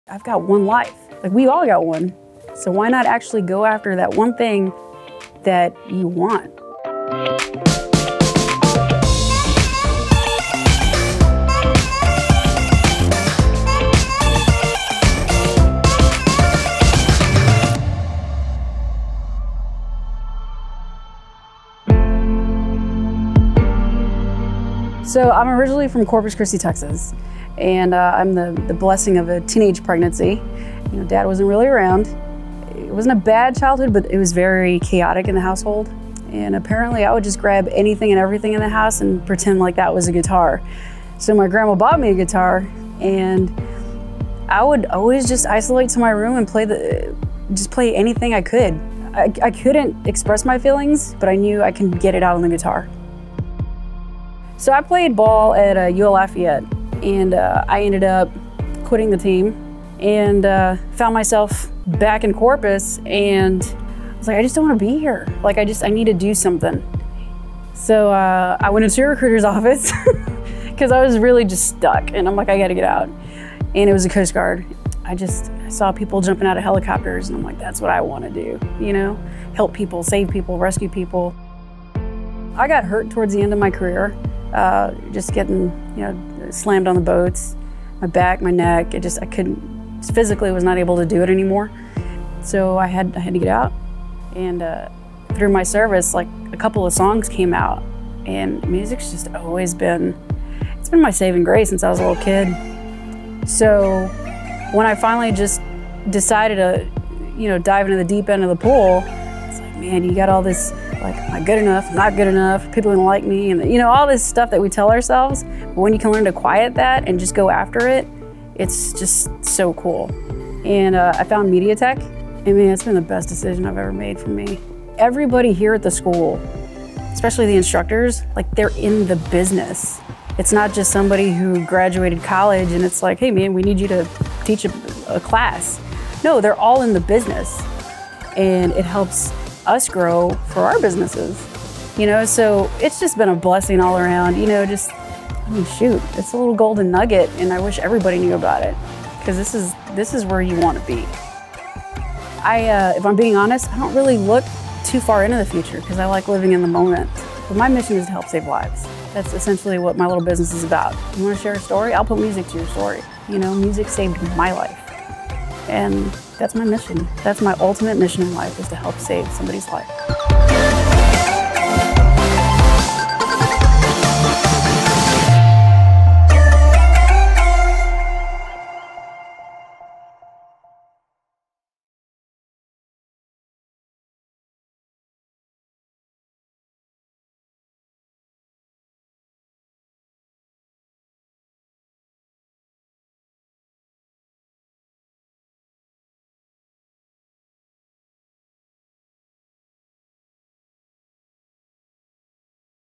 Producing Music Saved my Life. | Student of the Month
Pam Barragan is a MediaTech Recording Arts student, for which music has an enormous place in her heart. A Coast Guard Veteran, her main goal is to save people...whether that be by jumping out of helicopters or by producing their music.